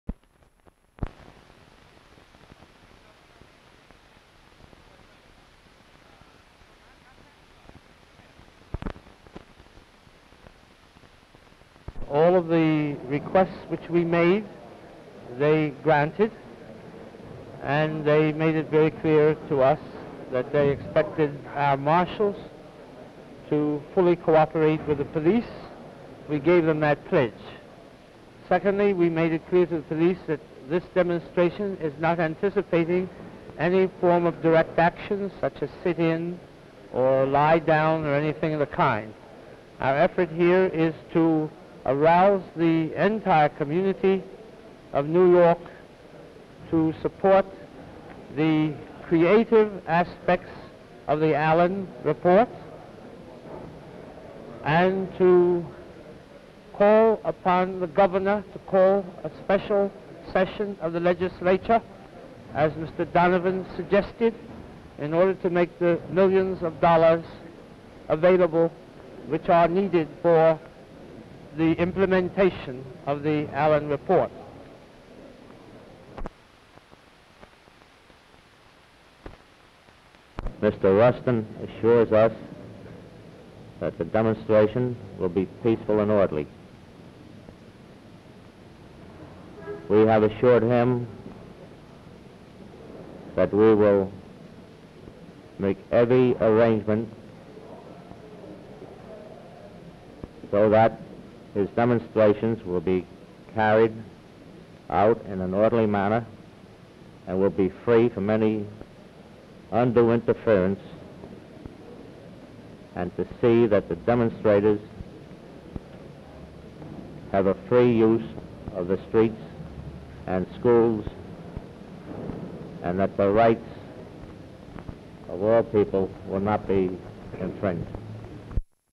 News Item (651df7c6-e58a-c238-ee4d-70f028a064ab)
(15 May 1964) 05/15/64   a0015276
 new york city:  civil rights leader bayard rustin met with city police and agreed an orderly demonstration on may 18th to support allen report recommedations:

 disc in ed - ny ny
 rustin, bayard - sof
 mc kearney, lawrence - sof

 xx allouf / 190 ft / 16 neg / d2781